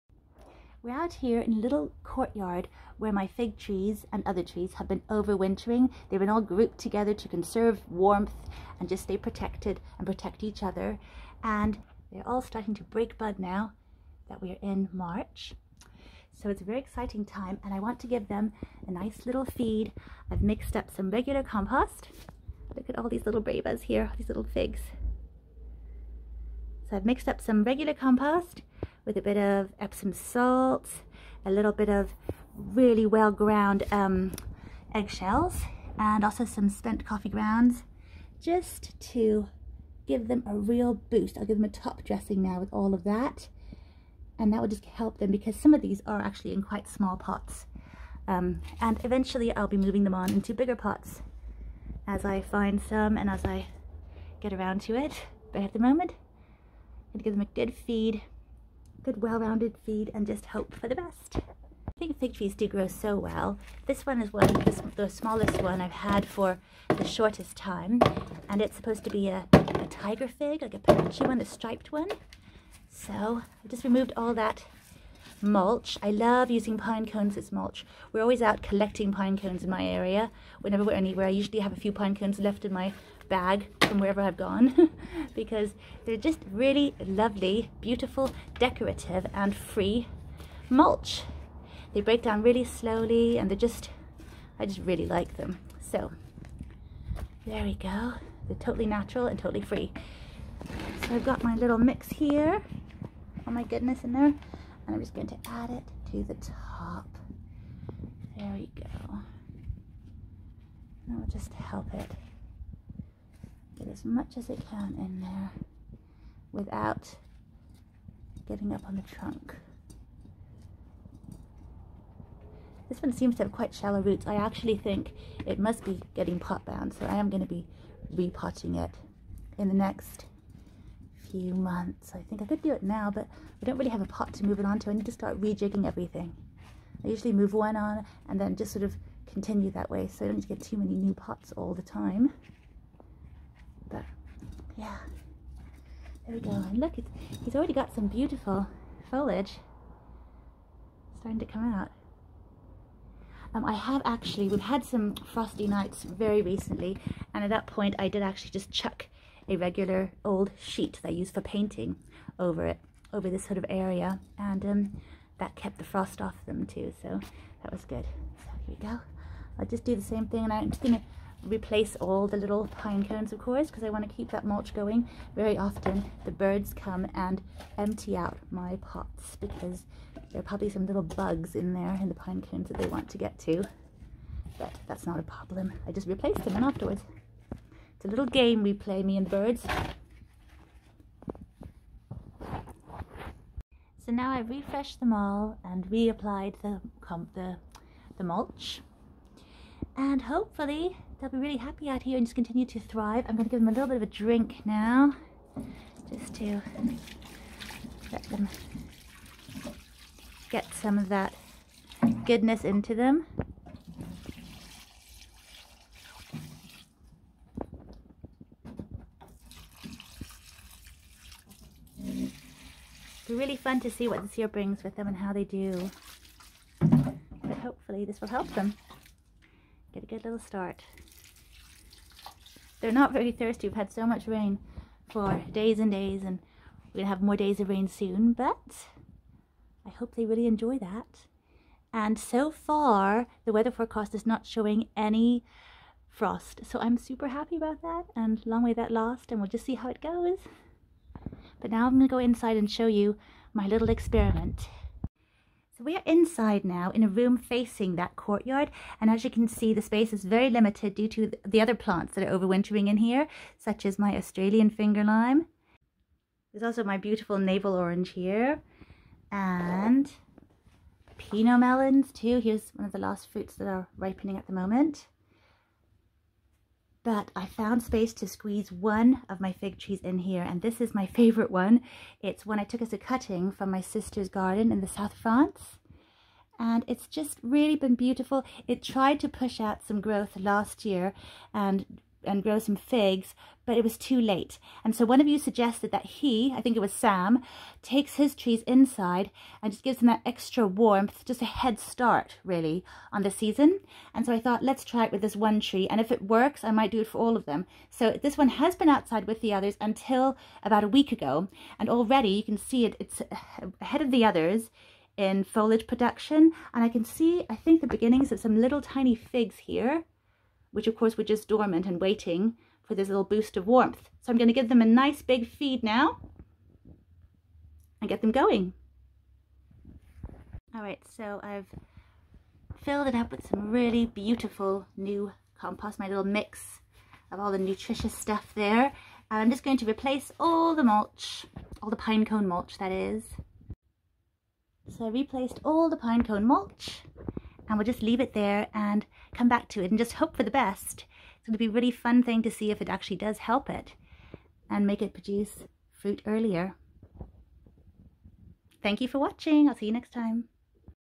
Feeding my potted Fig Trees and trying to push for early figs!
I so hope this works! This way I don't need to worry about late frosts either, which is a huge bonus.